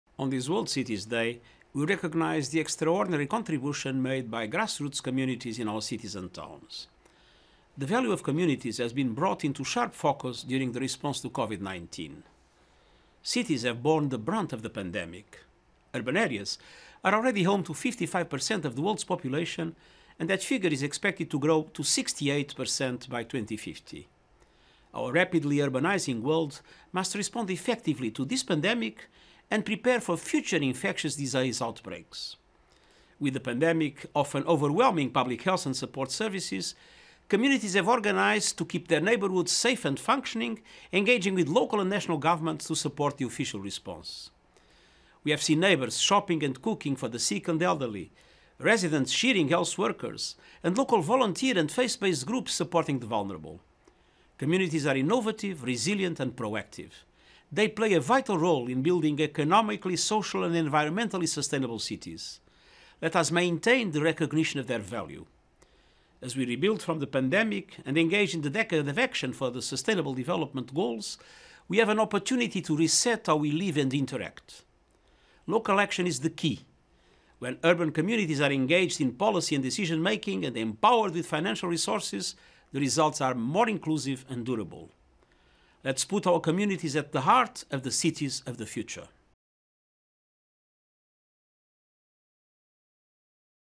Ação local é fundamental, diz chefe da ONU no Dia Mundial das Cidades
O secretário-geral das Nações Unidas, António Guterres, gravou uma mensagem marcando o Dia Mundial das Cidades (31) e reconhecendo a “contribuição extraordinária” feita pelas comunidades nas áreas urbanas. O valor das comunidades, disse, é ainda mais evidente na resposta à COVID-19.

“As cidades sofreram o impacto da pandemia. As áreas urbanas já abrigam 55% da população mundial e esse número deverá aumentar para 68% até 2050. O nosso mundo em rápida urbanização deve responder com eficácia a esta pandemia e se preparar para futuros surtos de doenças infeciosas”, destacou Guterres.

Com a pandemia fazendo grande pressão nos serviços de saúde pública e de assistência social, as comunidades se organizaram para manter bairros seguros e em funcionamento, trabalhando com os governos locais e nacionais para apoiar a resposta à COVID-19.

“Vimos vizinhos fazendo compras e cozinhando para pessoas doentes e idosos, residentes apoiando profissionais de saúde e grupos locais de voluntários e religiosos apoiando os vulneráveis”, exemplificou Guterres.

Na visão de Guterres, as comunidades são inovadoras, resilientes e proativas. “Elas desempenham um papel vital na construção de cidades sustentáveis em nível econômico, social e ambiental. Vamos continuar a reconhecer o seu valor.”

“Enquanto nos recuperamos da pandemia e nos envolvemos na Década de Ação para o Desenvolvimento Sustentável, temos a oportunidade de redefinir a forma como vivemos e interagimos. A ação local é fundamental”, disse Guterres, em referência aos dez anos que faltam para o prazo principal dos Objetivos de Desenvolvimento Sustentável (ODS), em 2030.

Quando as comunidades urbanas estão envolvidas na formulação de políticas e decisões, capacitadas com recursos financeiros, destaca a ONU, os resultados são mais inclusivos e duradouros.

“Vamos colocar as nossas comunidades no centro das cidades do futuro”, destacou António Guterres.

(Foto de capa do vídeo: Melen, favela na capital de Camarões, Yaoundé. Crédito da foto: ONU-Habitat/Kirsten Milhahn)